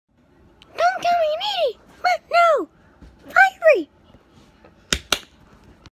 Needle voice
I don’t know how to make a needle voice but I’m going to make a firey voice